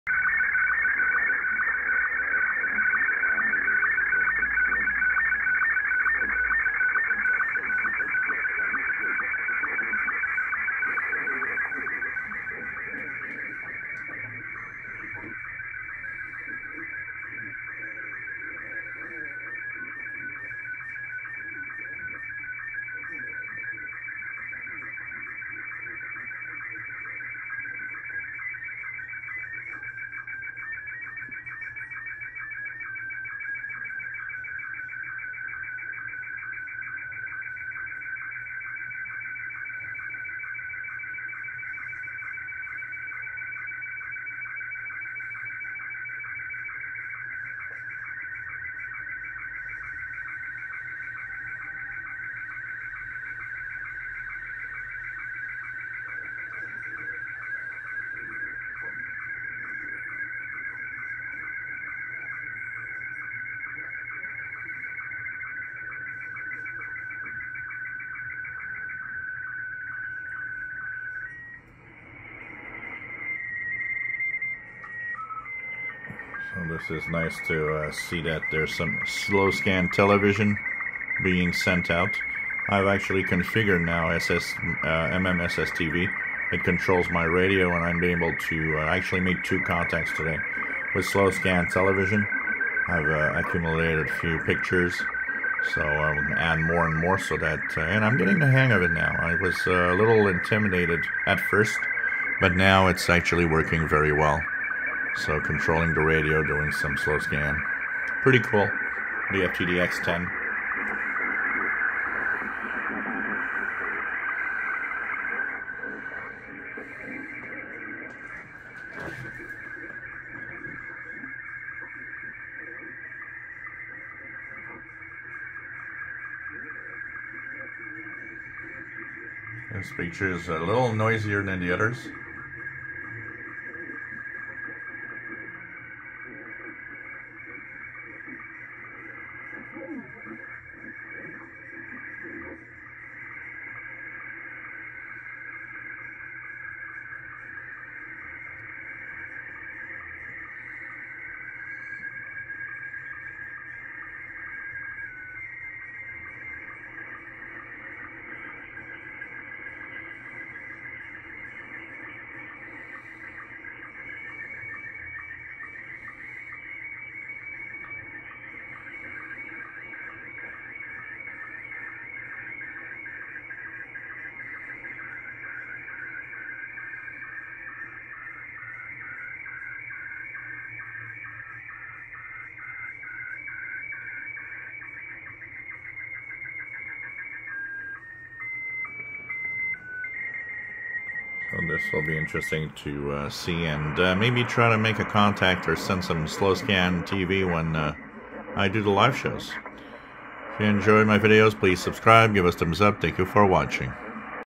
MMSSTV configured with Yaesu FTdx10 ready to send and receive Slow Scan Television pictures
Did a few contacts today and having fun with SSTV